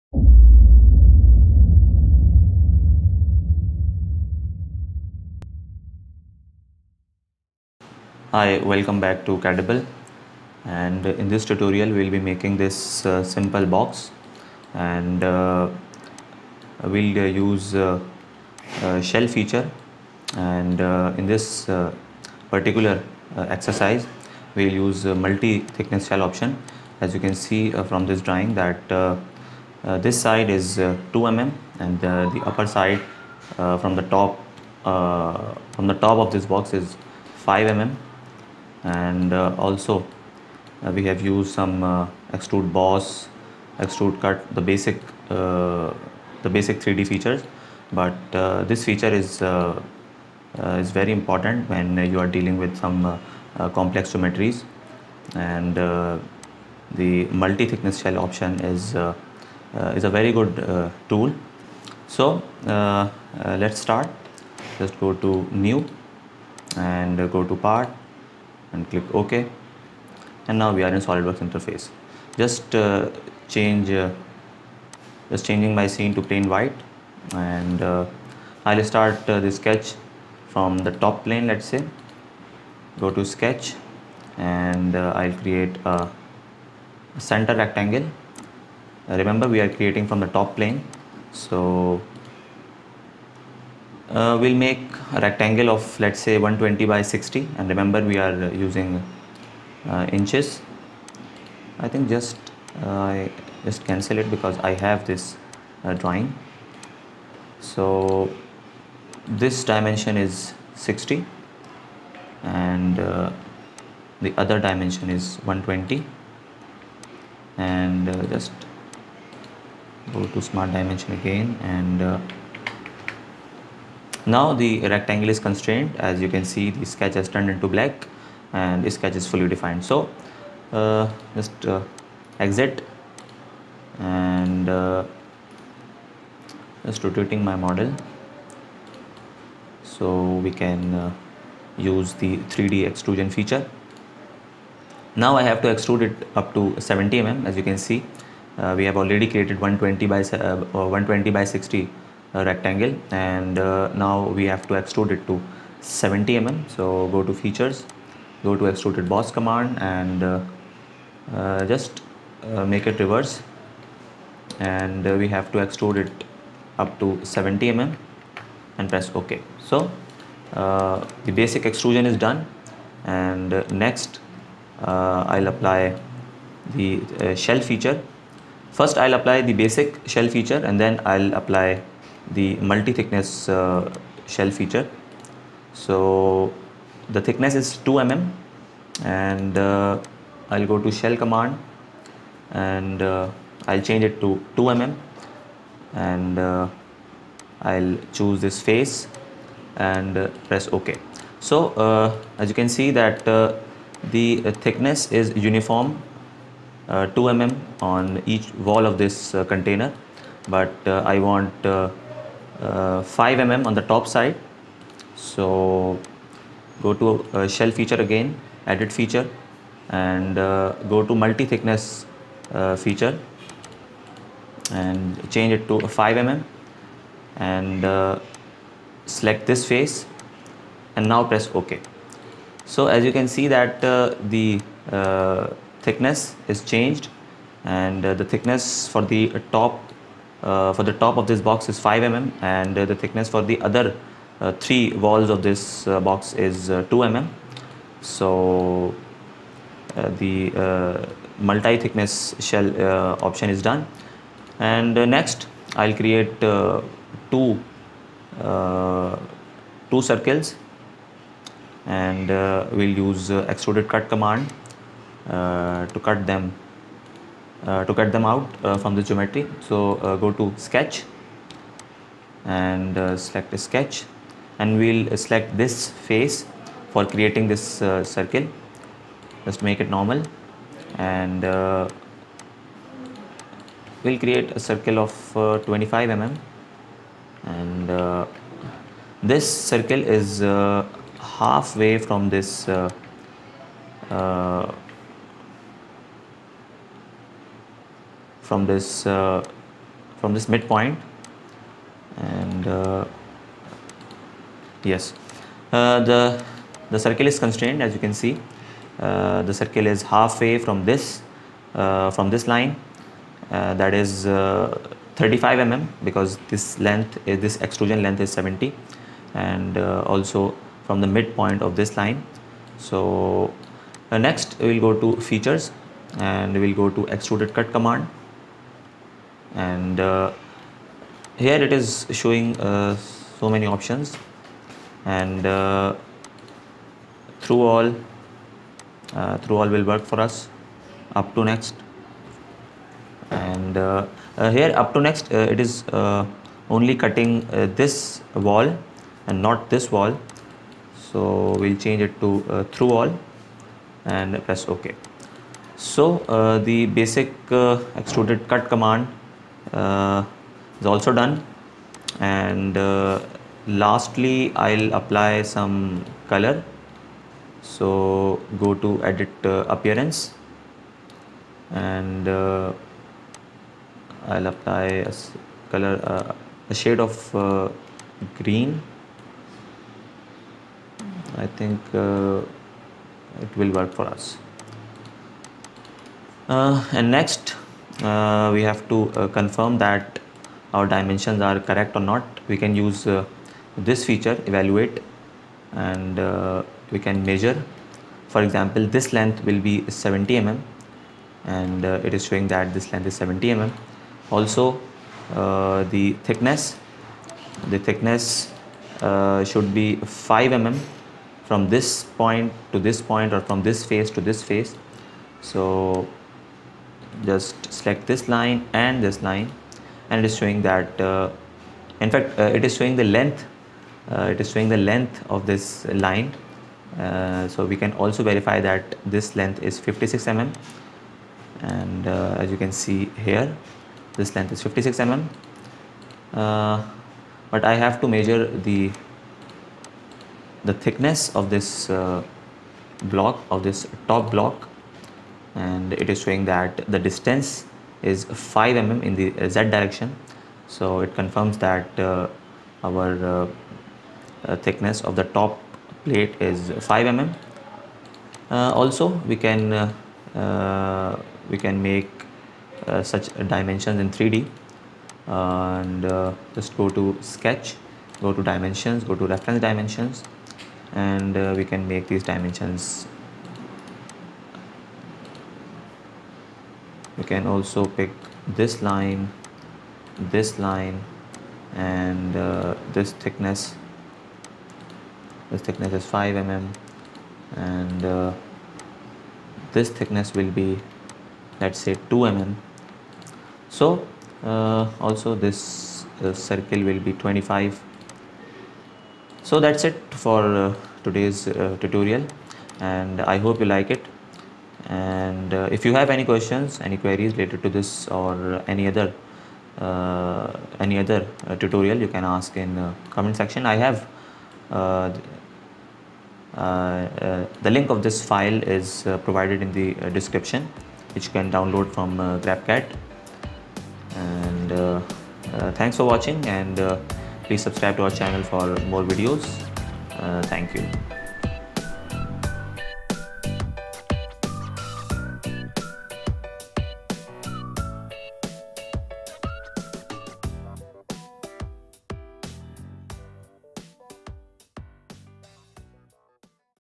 Solidworks Exercise 4 | Solidworks Multi thickness shell | CADable | Solidworks Mechanical Design
Solidworks Exercise 4 |  Solidworks Multi thickness shell | CADable | CADable tutorials | Solidworks practice exercises

Hi welcome back to CADable. Here I am starting a new playlist related to basic tutorials of SOLIDWORKS. This is the 4th tutorial of this playlist. It is a good exercise for beginners and it will be a good practice for beginners.
In this tutorial, we will learn these commands:

1) Basic 2D Commands
2) Extruded Boss
3) Shell Command
3) Multi-Thickness Shell
4) Color
5) Evaluate
6) 3D Dimensions

Kindly use play pause technique for practice. I have attached files in the description for practice. I will try to answer each and every question. Keep practicing as practice makes a man perfect.